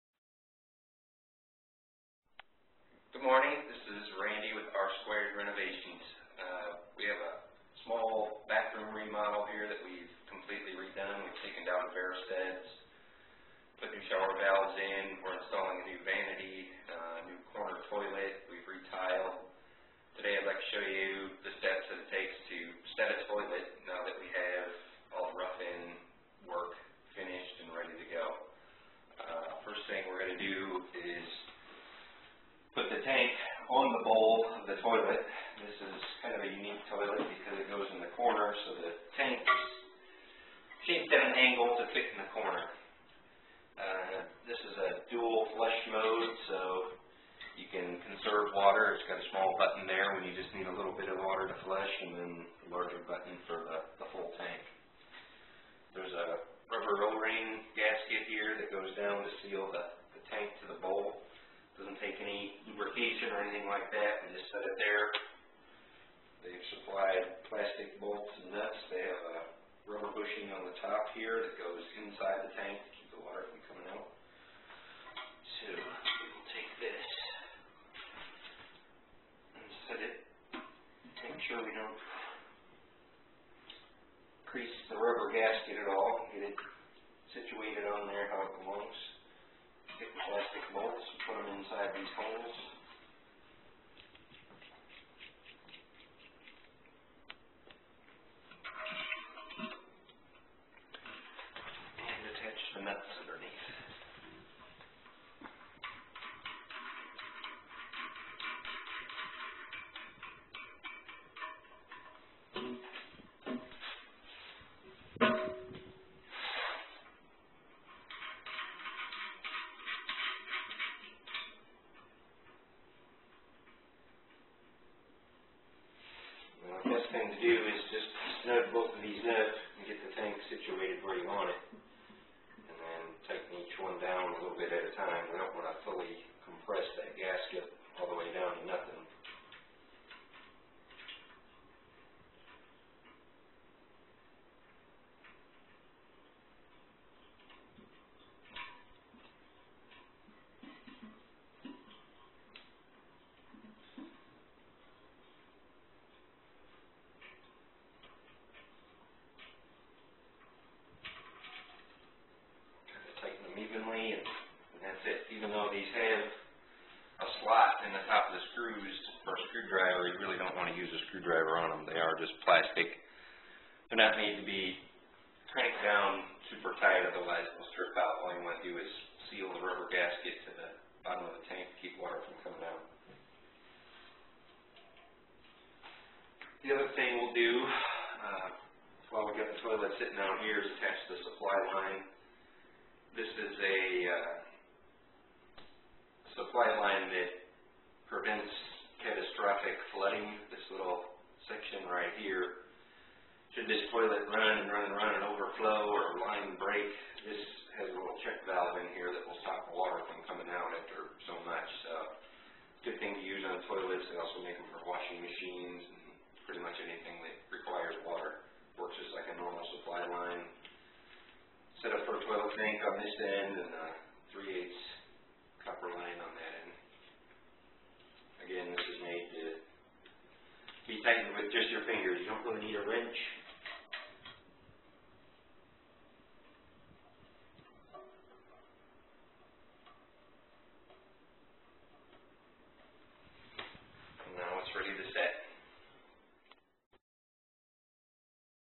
Toilet Installation, Part 1 with Randy from R Squared Renovations
a General Contractor in Dallas, TX demonstrates the simple steps involved in toilet installation or replacement.  This video is ideal for the Do It Yourselfer (DIY).  This is one of a series of videos to help home owners with their bathroom remodel project.  Enjoy!

Transcription: Good Morning, I'm Randy with R Squared Renovations.  We have a small bathroom remodel here that we've completely redone taken it down to bare studs.  We've put new shower valves in a new vanity and a new corner toilet that we've retiled.  Today I'd like to show you the steps that it takes to set a toilet now that we've all the rough end work finished and we're ready to go.

The first thing we're going to do is put the tank on the bowl of the toilet.  This is a unique toilet because it goes in the corner so the tank is shaped at an angle to fit in the corner.  This is dual flush mode so you can conserve water its got a small button when you just need a little bit of water to flush or a large button for when you need a full tank.

There's a rubber o ring gasket that seals the tank to the bowl.  Doesn't need any lubrication.  The supply plastic bolts and nuts.  They have a rubber cushion on the top that goes inside the tank so the water doesn't come out.  We'll take this and set it, make sure you don't crease the rubber gasket at all get it situated on there how it belongs.

Take the plastic bolts and fit them in these holes. And attach the nuts underneath. The best thing to do is get them pretty sung then once you fit it tighten them down a little at a time.  You don't want to fully compress the gasket all the way down to nothing.

Tighten them evenly and thats it.  Even though these have a slot on the top of the screws to use with a screwdriver, you really don't want to use a screwdriver.  They are plastic.  They are not made to be cranked down super tight otherwise it will start battling with you as seals and the rubber gasket and the bottom of the tank keep the water from coming out.

The other thing we will do is attach the supply line.  This is a supply line that prevents catastrophic flooding.  It has a little section right here.  Sound this toilet run and run and run and overflow or a line break, this has a little check valve that will stop the water from coming out after so much.  Good thing to have on a toilet.  They also make them for washing machines and pretty much anything that requires water.  it works like a normal supply line.

It's setup for a toilet tank on this end and 3/8ths copper line on that end.  Again this is made to be tightened with your fingers.  You don't need a wrench. And now it's ready to set!